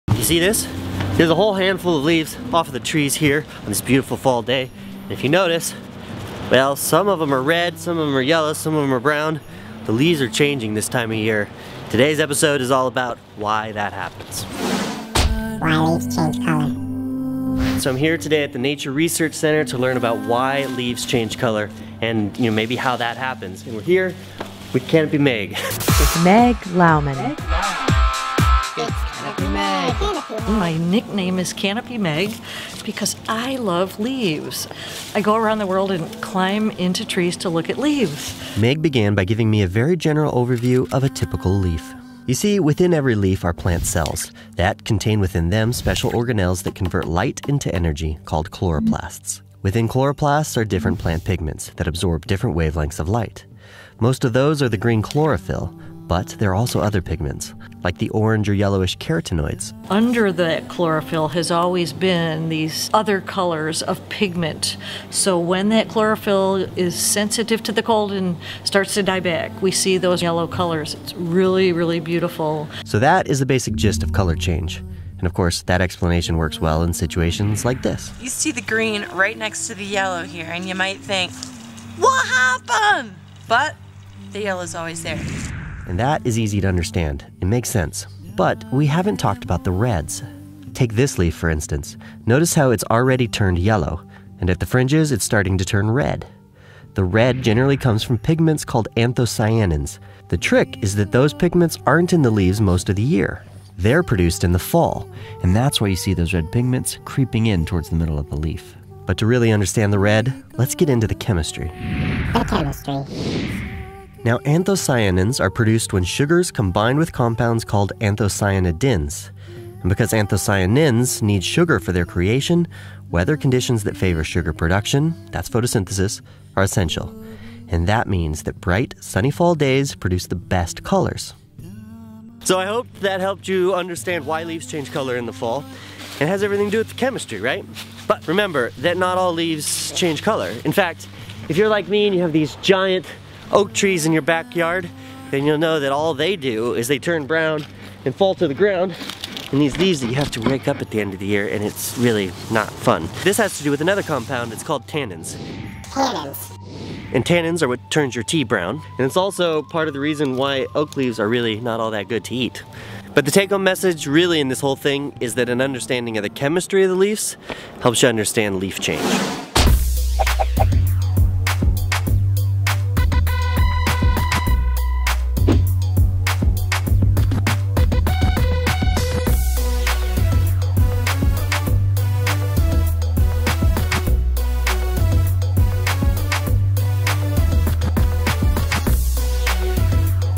Why Leaves Change Color: Untamed Science
Every fall in the deciduous forests of the world, there is a miraculous change in the color of the leaves. They turn from green into brilliant shades of yellow, orange, red, and purple. Sometimes you can see several shades on the same leaf. But why are these color changes happening, and what is going on in the plant?

Fortunately, that's the topic of this Untamed Science video. Rob explains, with the help of Canopy Biologist, Meg Lowman, that it has everything to do with the pigments in the leaves. In fact, many of those pigments have been their all year and can only now be seen. Others, like the red color are a result of pigments produced in the plant as it dies.